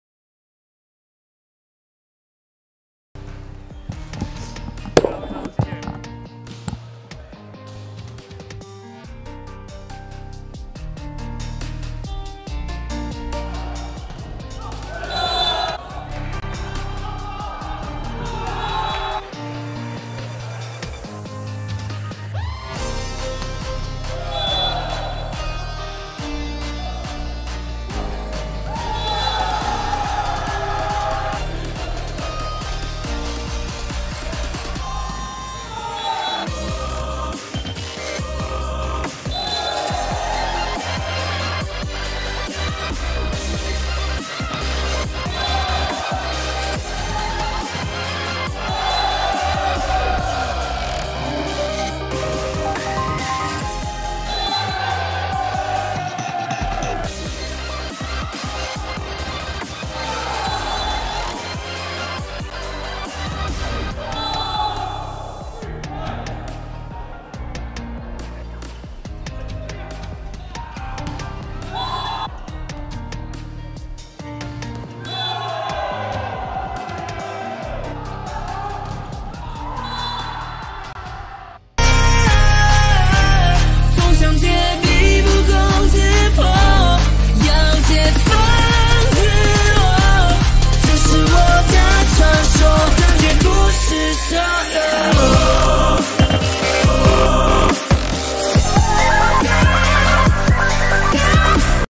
明德高中 vs 頭城家商全場精華 Volleyball Full Game Highlights
1️⃣22:25
2️⃣21:25
3️⃣21:25
📌📋頭城家商以局數3:0拿下本場比賽勝利
2023登峰造極賽程比分